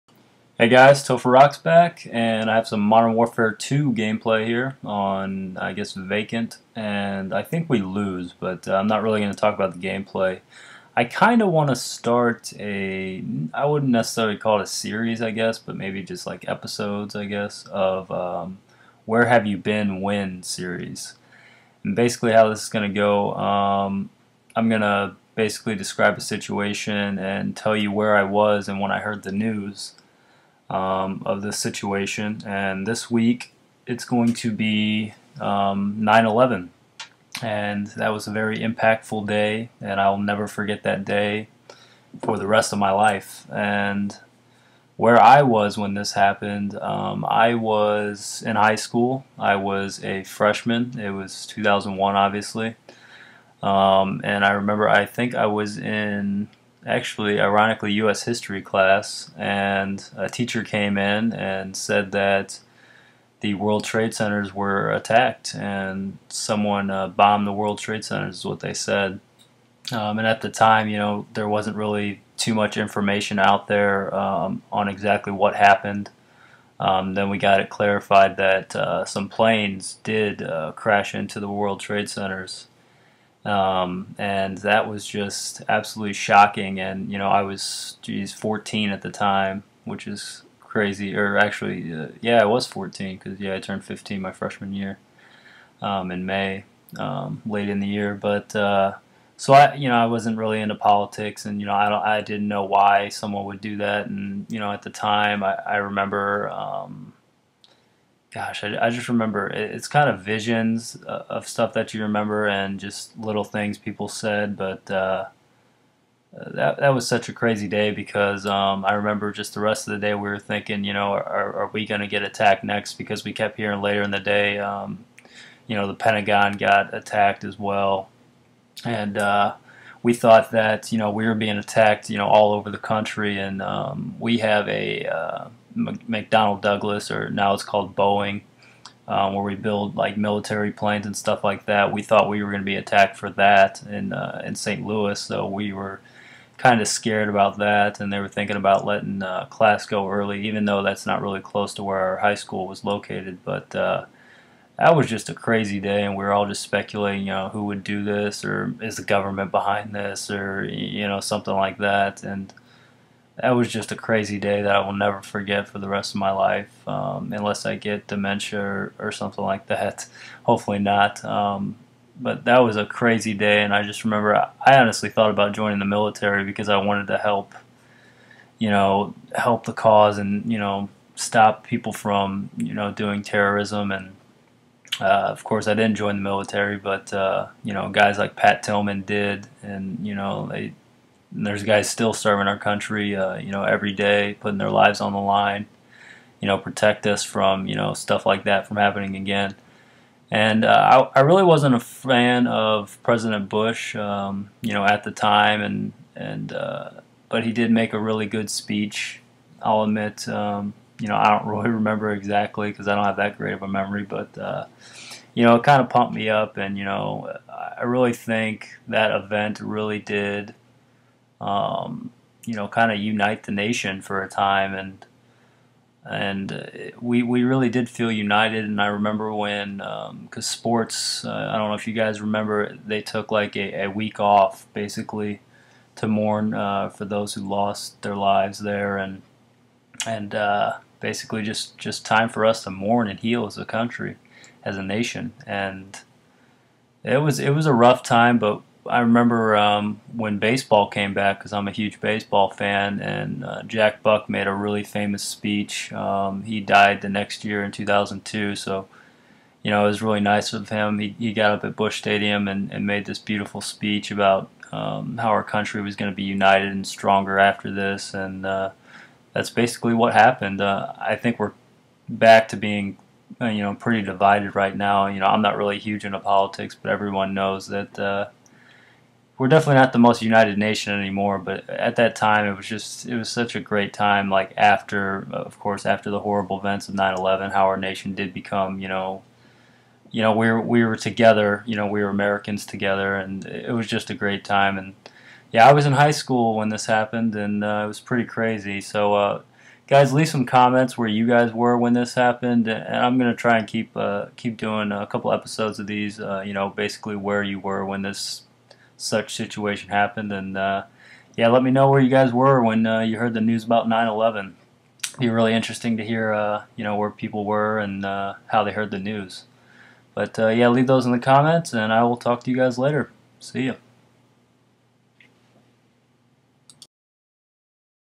Where Were You? Episode 1 (9/11)
Where were you on September 11, 2001? I explain where I was and how I found out about the attacks on the World Trade Center.